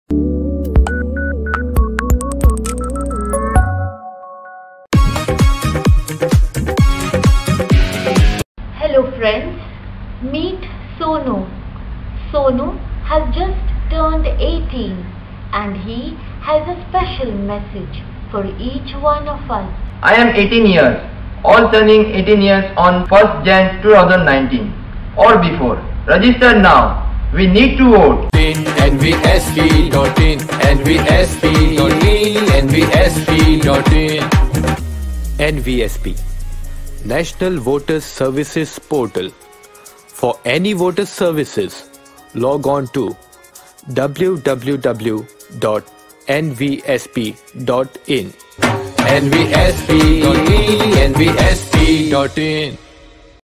Turning 18 on 01/01/19 Register as Voter today
Turning 18 on or before 01/01/2019, you must register as voter. Apply online at nvsp.in